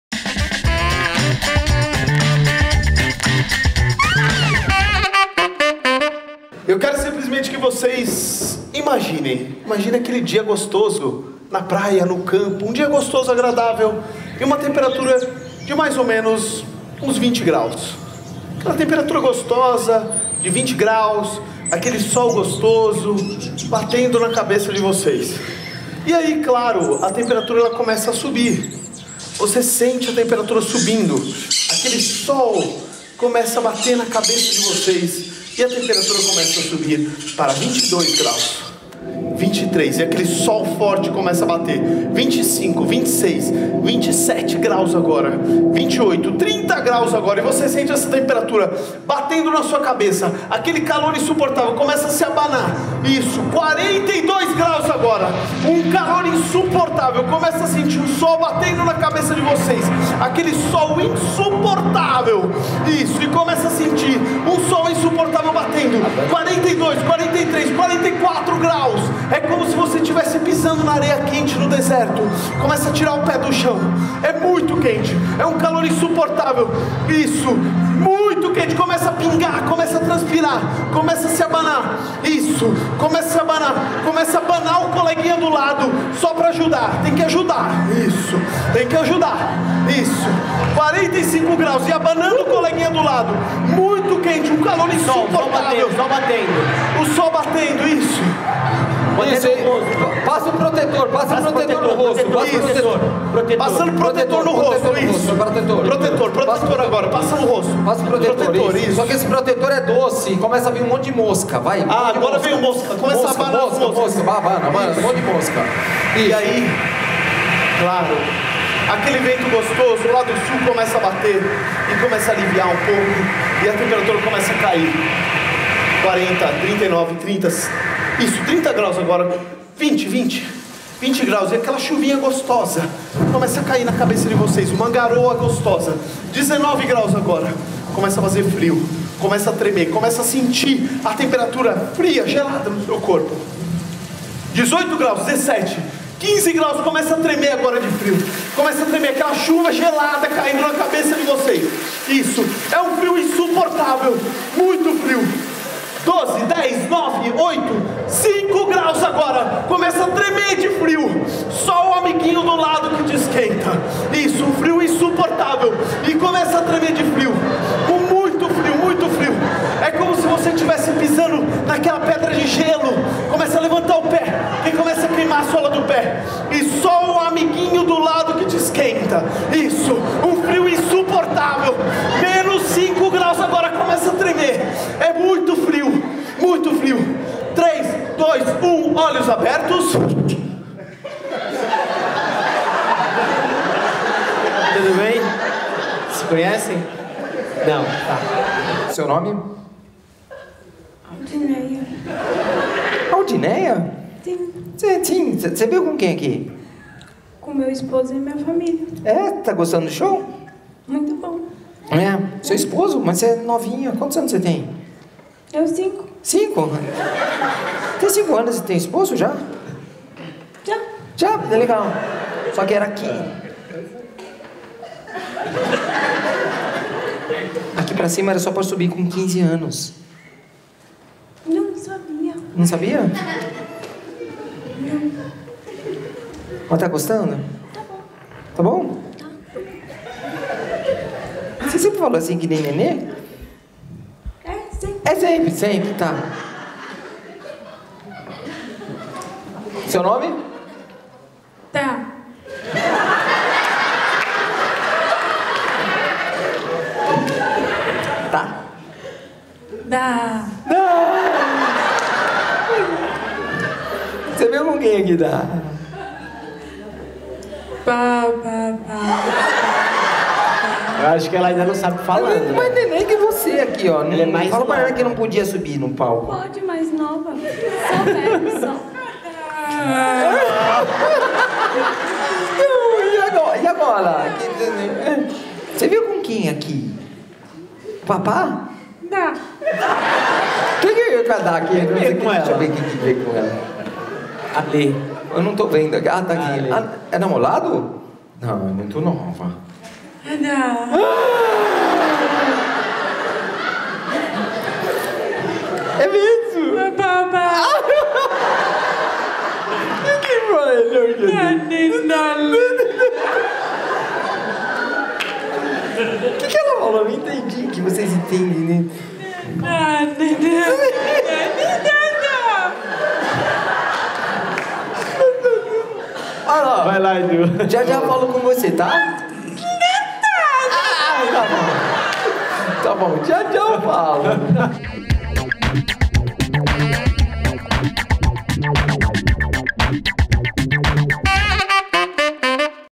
HIPNOSE - UMA TINHA 05 ANOS E A OUTRA... QUANTOS ANOS? (26/05/18)
Em nossa apresentação do dia 26/05, tivemos mudanças drásticas de temperatura no palco... Aliás, duas participantes não poderiam subir ao palco, pela idade: uma tinha 05 anos e a outra... Ninguém sabe !!

2018 - Janeiro à Abril - Todos Sábados às 20h